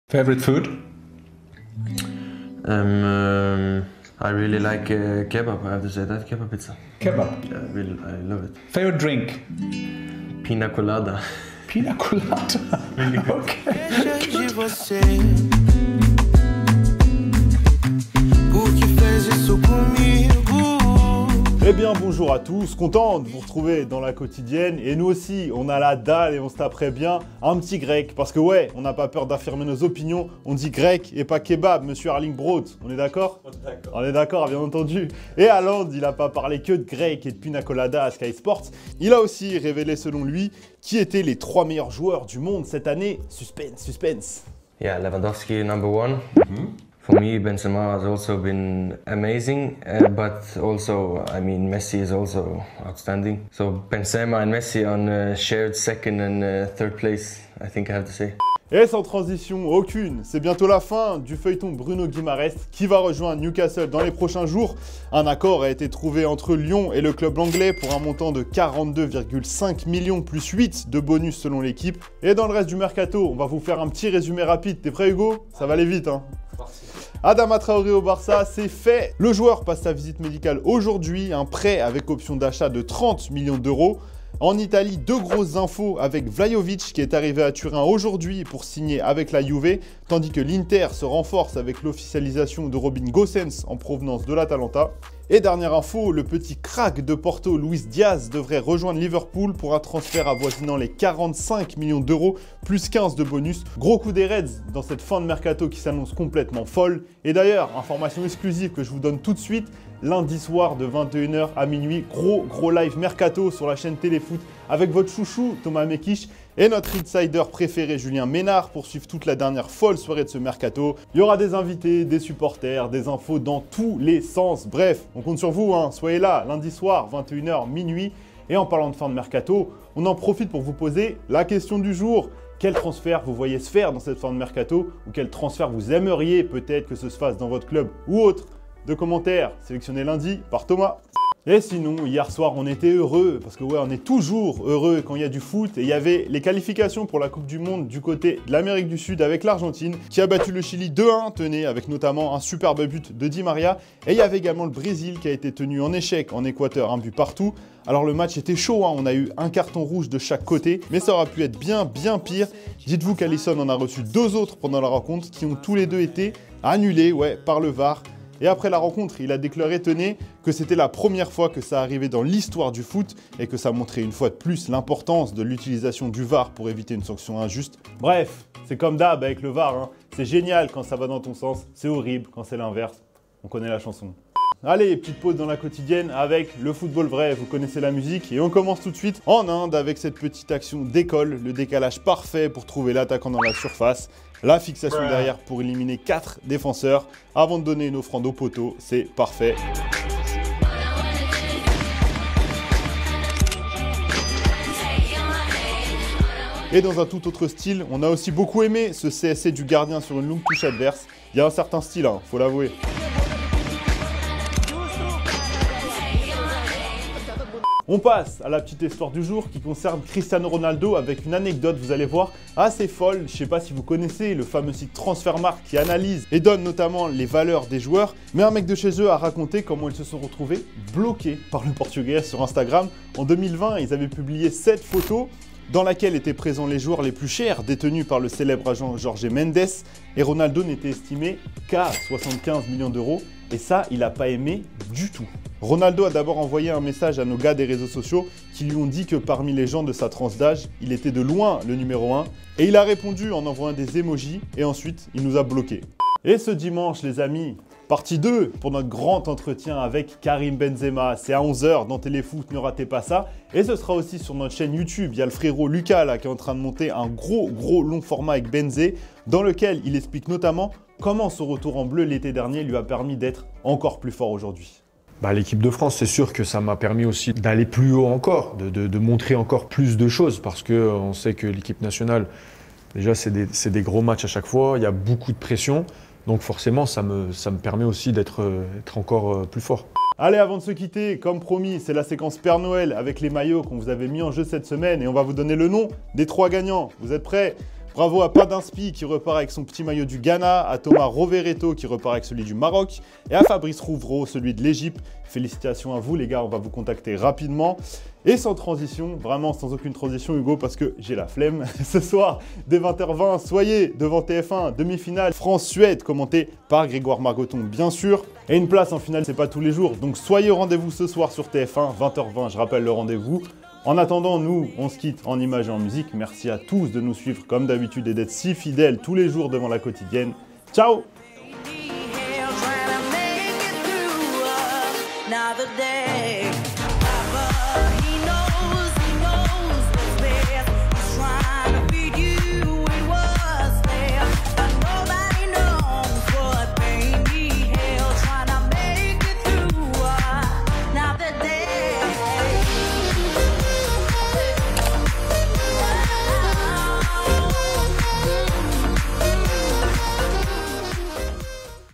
Cristiano Ronaldo RAGE et BLOQUE après avoir découvert combien il coûte ! - La Quotidienne #1011
Le coup de sang de Cristiano Ronaldo, Haaland donne son podium des meilleurs joueurs du monde, un point sur le mercato d'hiver : la Quotidienne du jour est  disponible !